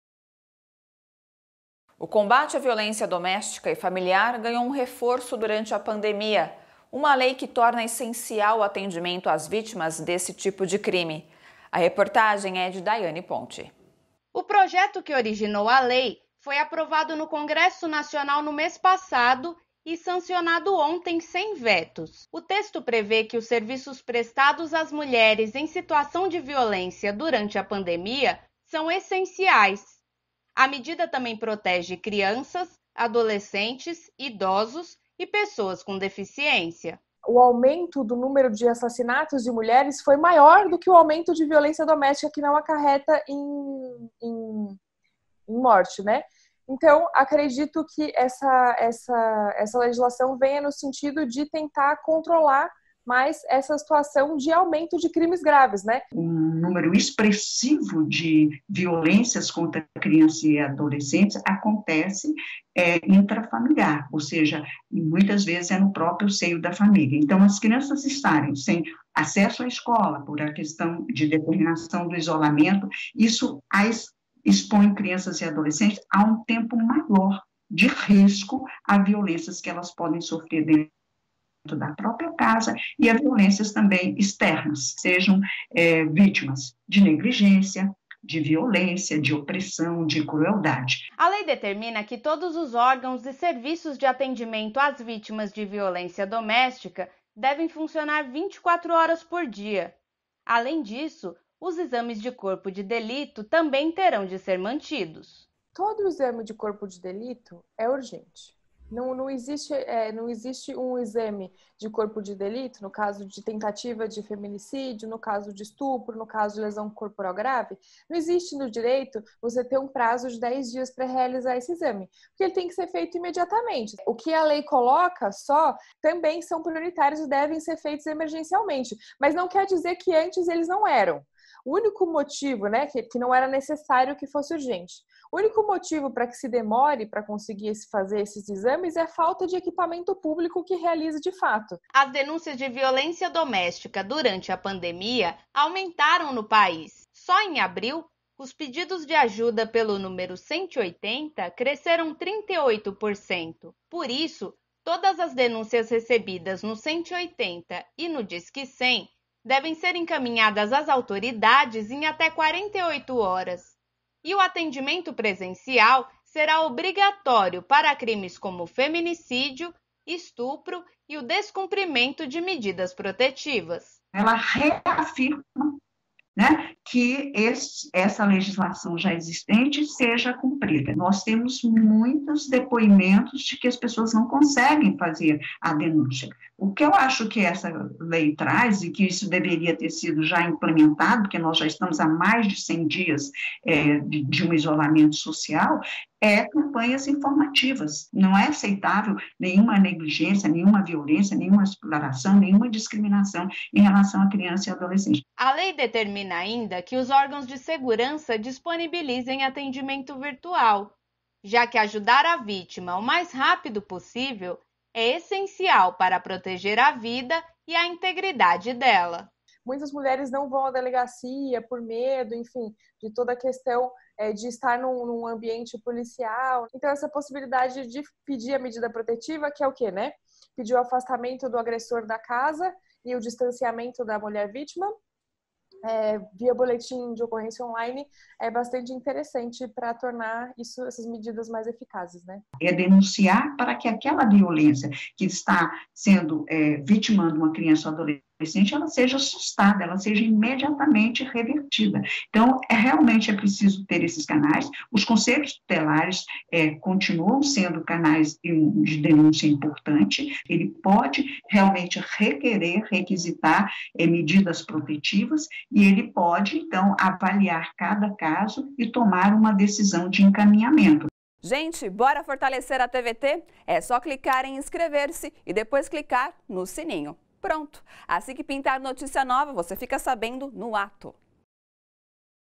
Lei torna essenciais serviços de combate à violência doméstica
O combate à violência doméstica e familiar ganhou um reforço durante a pandemia. Uma lei que torna essencial o atendimento às vítimas desse tipo de crime. O projeto que originou a lei foi aprovado no Congresso Nacional no mês passado e sancionado sem vetos.